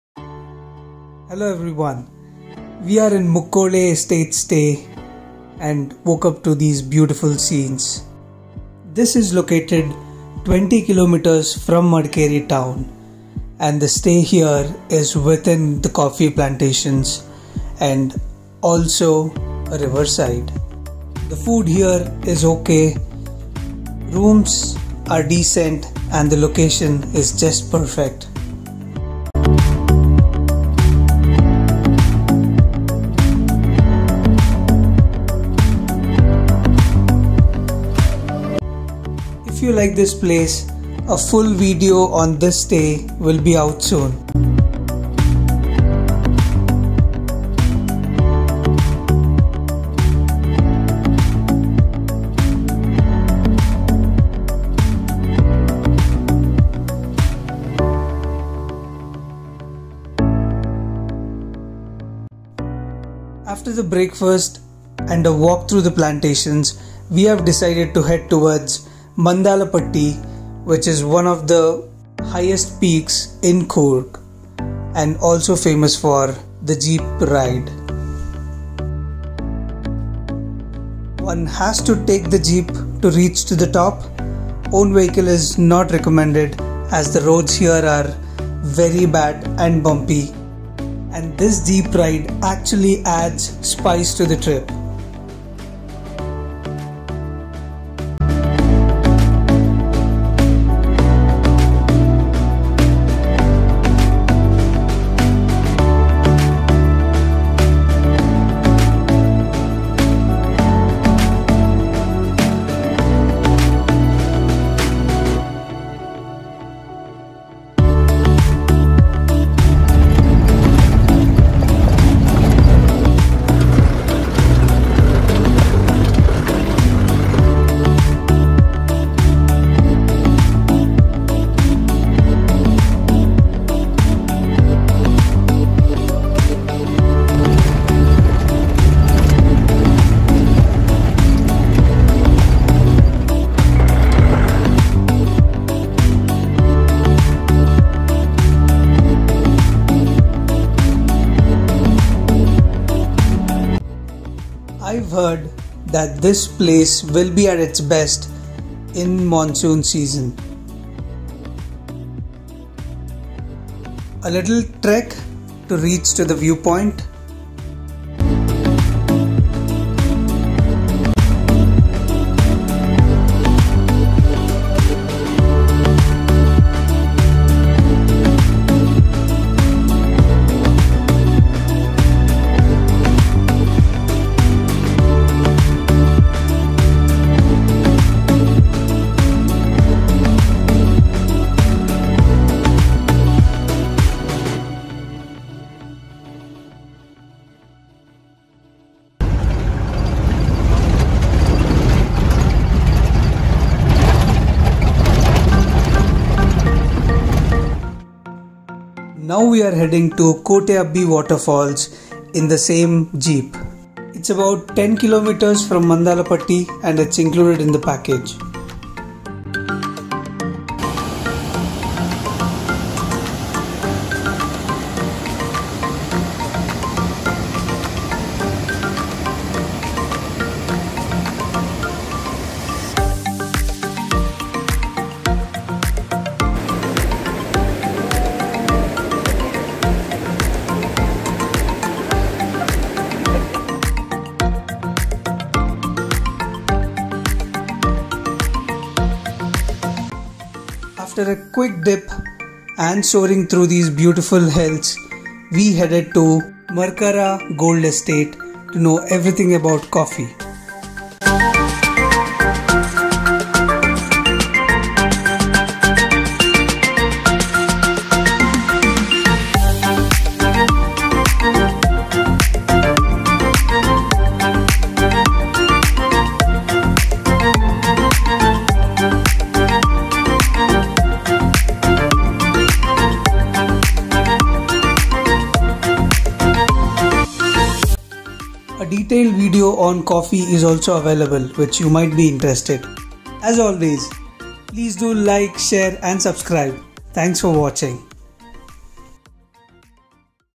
WoW - Trippin Coorg // Day 2 // Mandalpatti - Kote Abbi Falls - Mercara Gold Estate
This is a glimpse of journey with friends, in Coorg.
Visited Mandalpatti view point and Kote Abbe Waterfalls in a bumpy jeep ride.
Also, been to Mercara gold estate which is a Coffee plantation offering Coffee tours to show the process from Seeding to Growing to Roasting to Blending to Preparation. Includes an in-house store too, for buying Spices and Coffee.